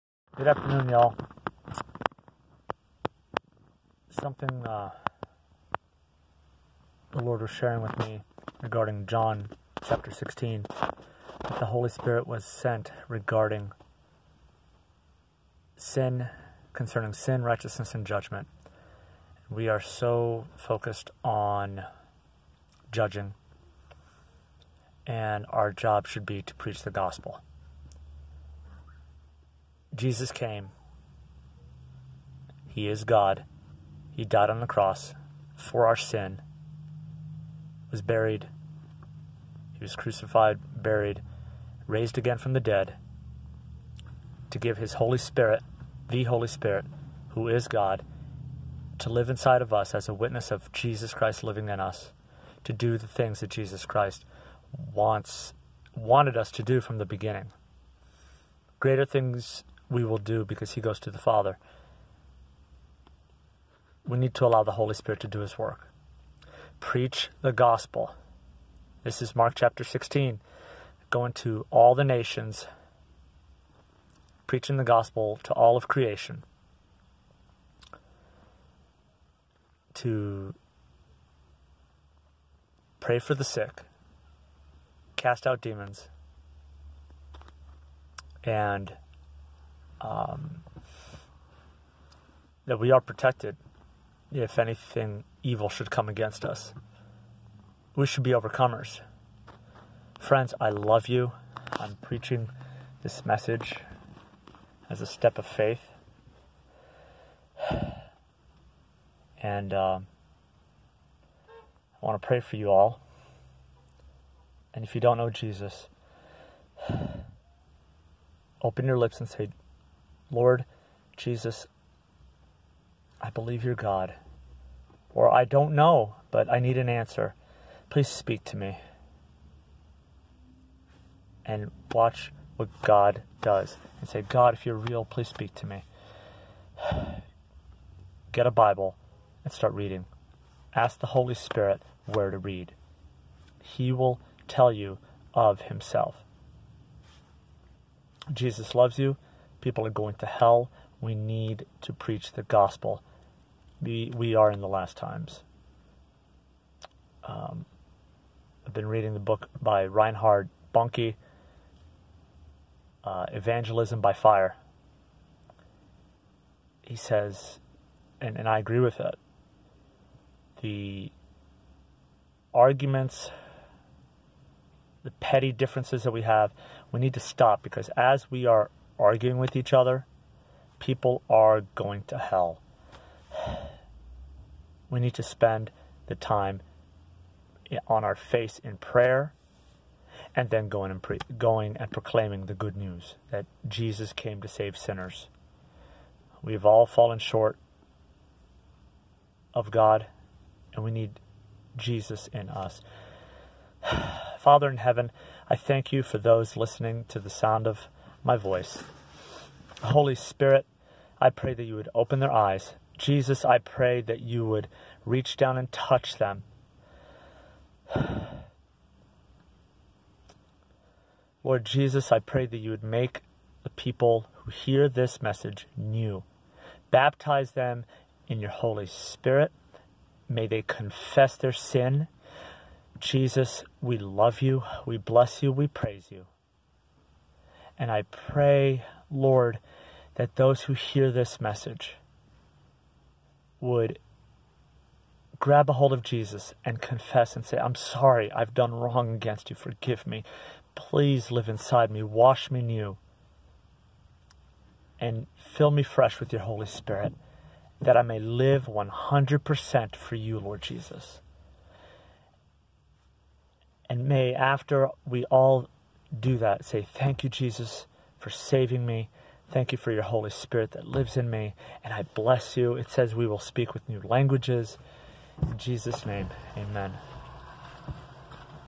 Preach Jesus Christ and Him crucified. Let the Holy Spirit do His job.
A message of faith, hope, and love. If the Holy Spirit has done a transformative work in you, please feel free to post the good news that He has done! Please be kind with your comments. Only unifying, building up, and enriching comments will be allowed.